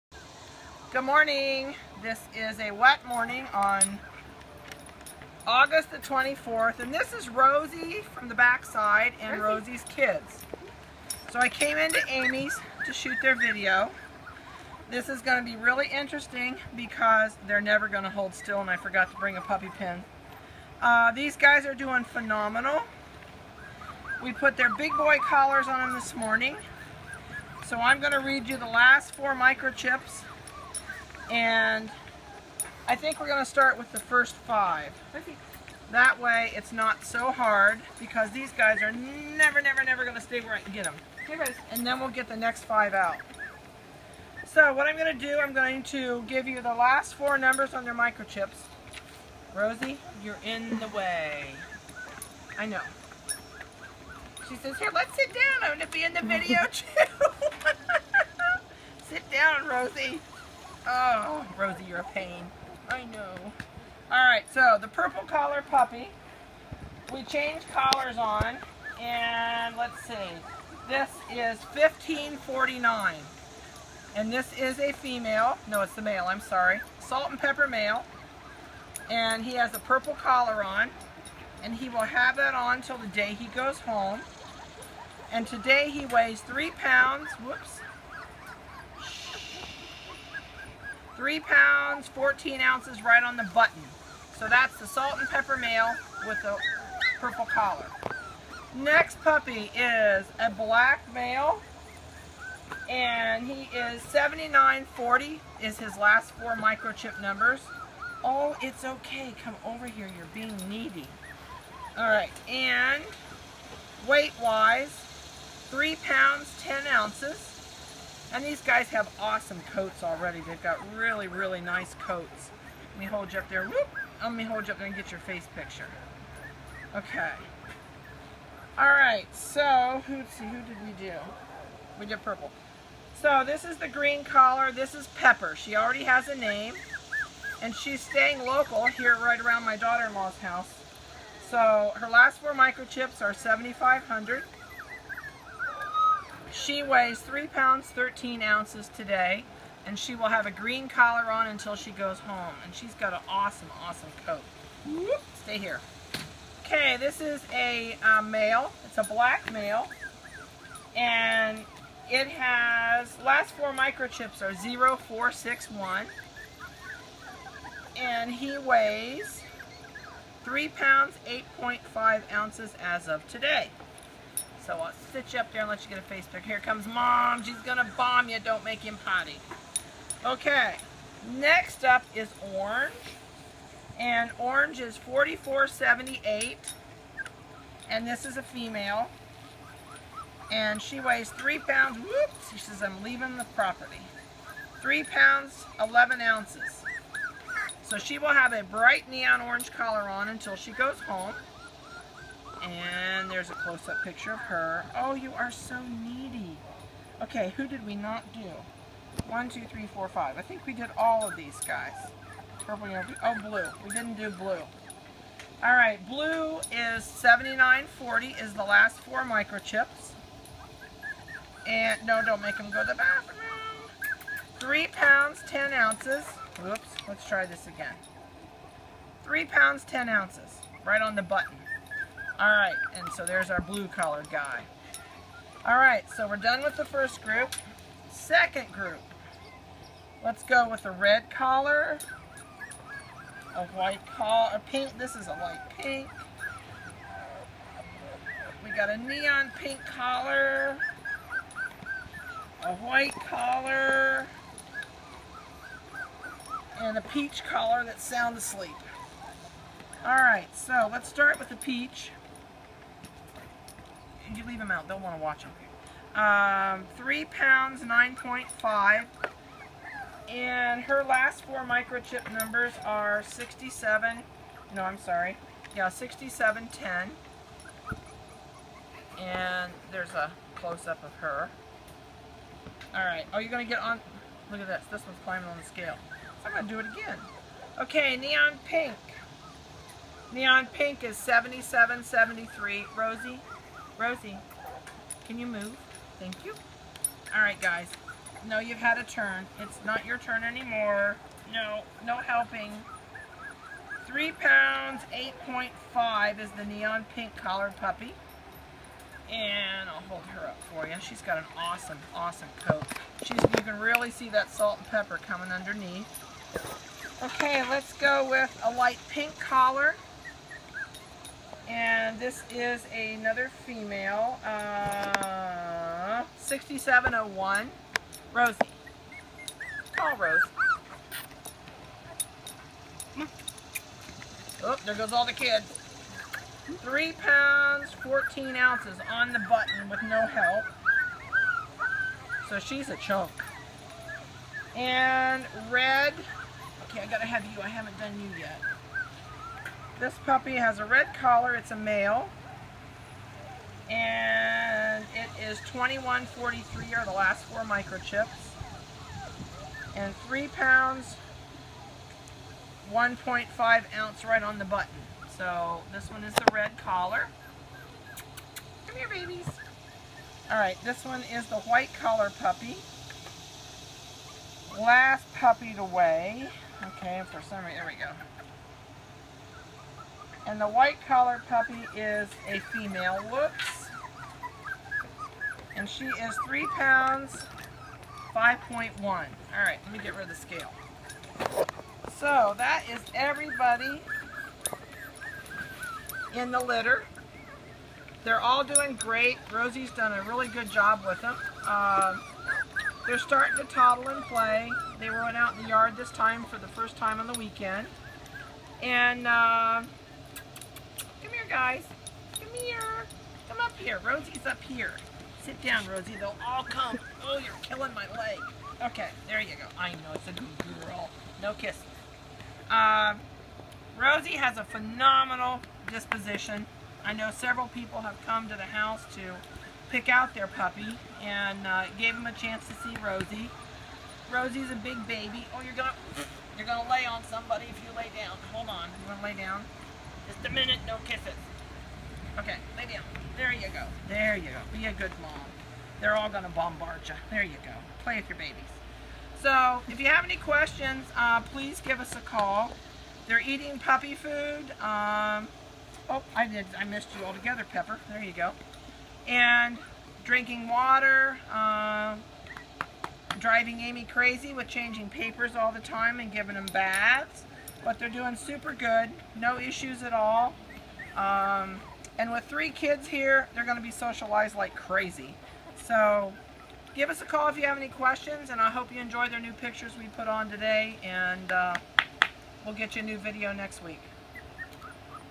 Rosies Standard Schanuzer puppies 8-24-15 4 weeks old tomorrow
There are 3 puppies still available in this litter and we have 2 other litters that were just born.